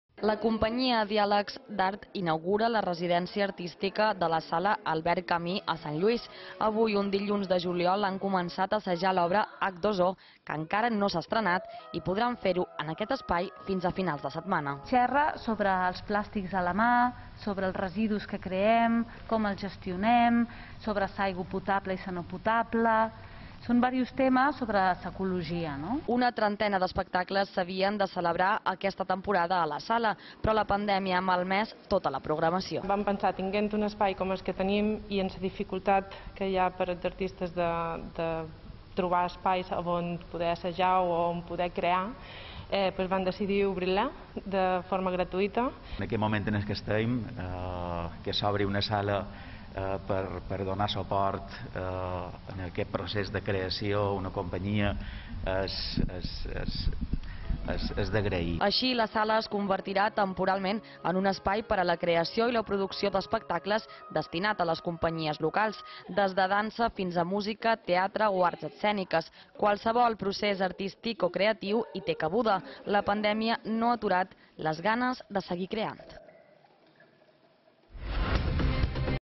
La sala Albert Camus de Sant Lluís s’obre gratuïtament com espai d’assaig i creació
Sant Lluís, a Menorca, ha obert la Sala Albert Camus per als artistes de l’illa. La pandèmia ha cancel·lat tota la programació de la sala d’aquesta temporada, i mentrestant s’ha decidit aprofitar aquest espai com a residència artística per a les companyies de música i teatre. L’objectiu: fomentar noves produccions culturals d’àmbit local.